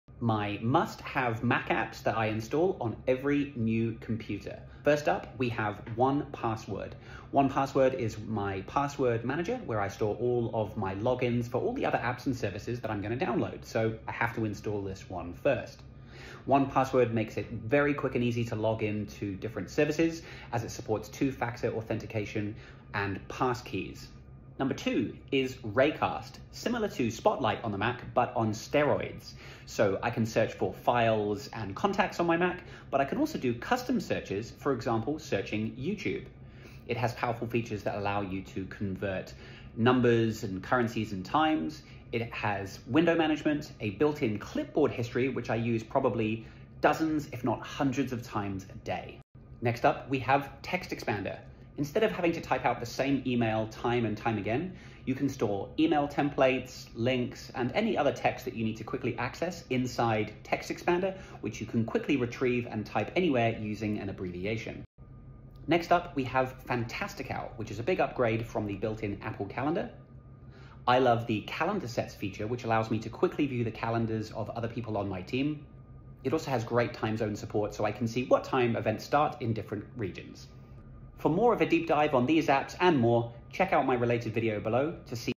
Must Have Mac Apps
Just got a new Mac? Or looking to give your old one a refresh? These are my must-have Mac apps I install on every single new Mac I own.